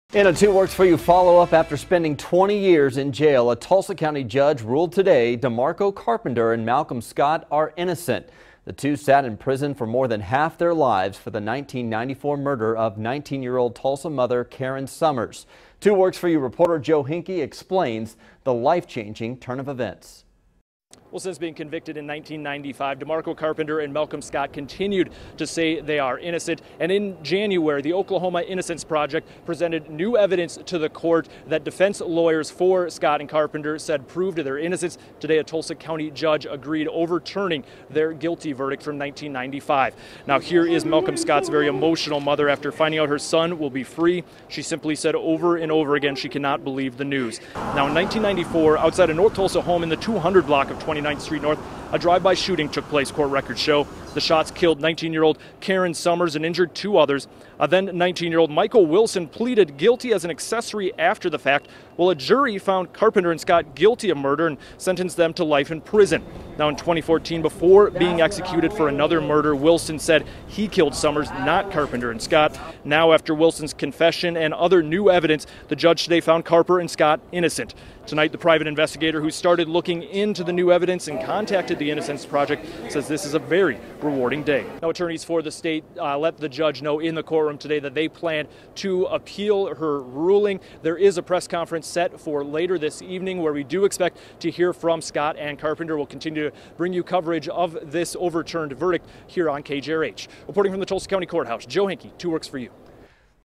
Men found innocent in 1994 drive-by shooting
Two men were found innocent Monday in a fatal 1994 drive-by shooting case.